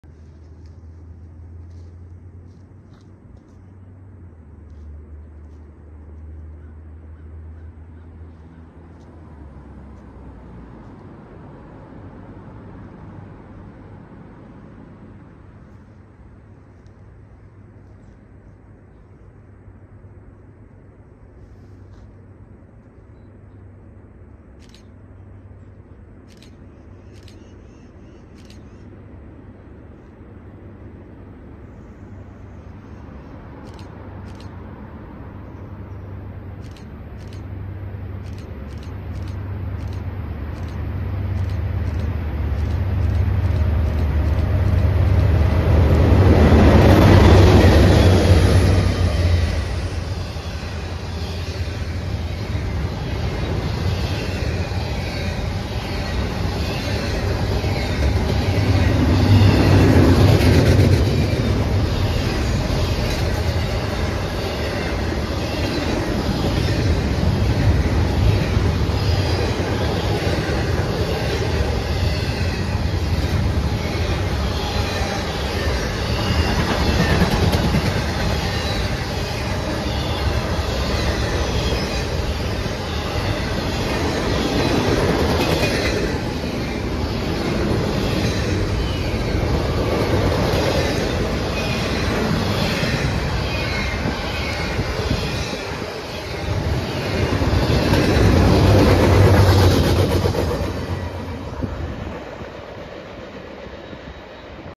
A very smoky WB NS Intermodal at Towners woods, Ohio
11/8/22
Ravenna, Ohio